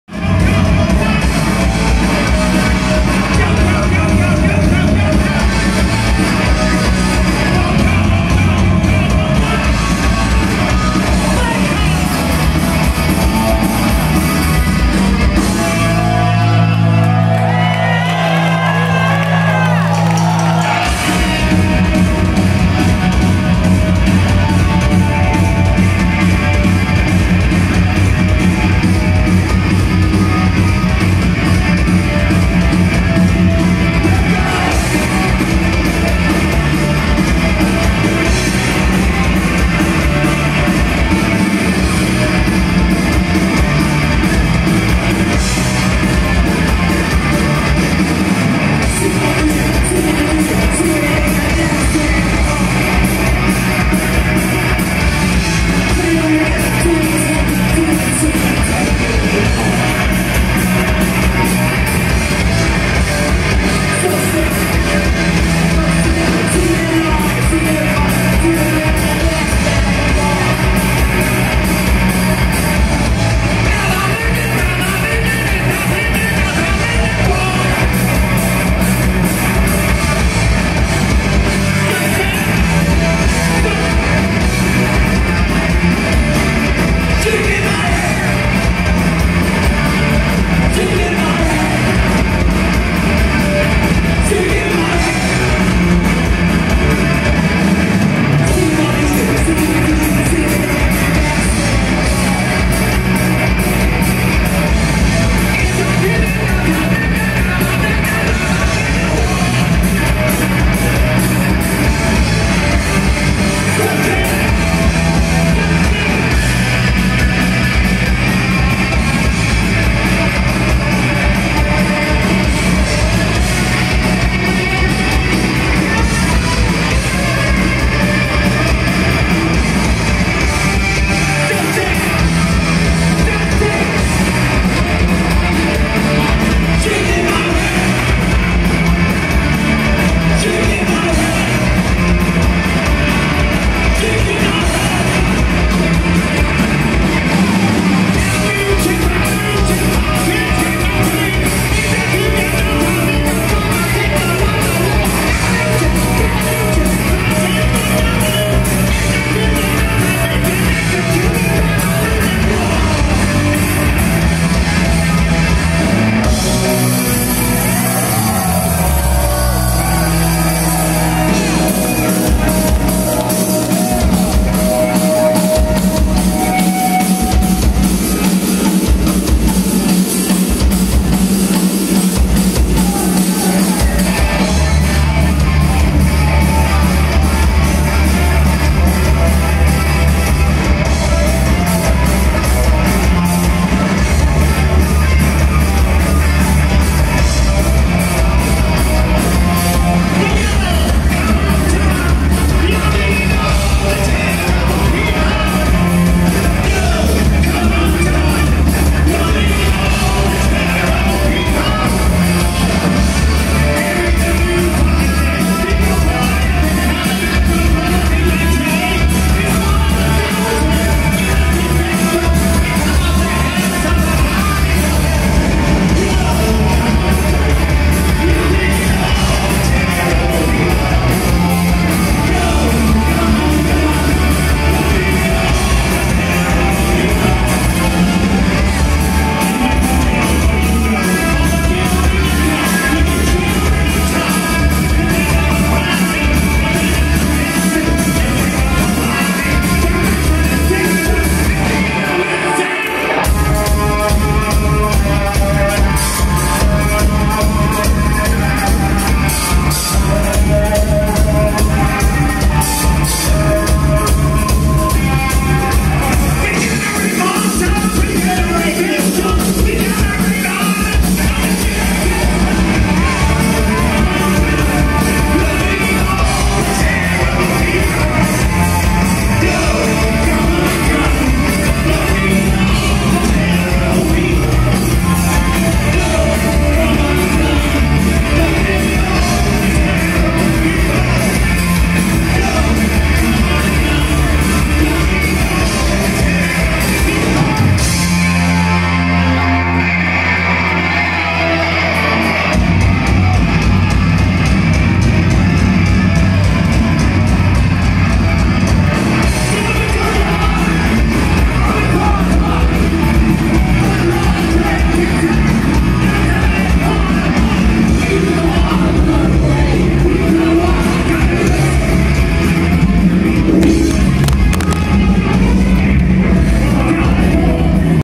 TSOL @ Remember the Punks Music Festival
Remember the Punks Music Festival in San Antonio 10/1/16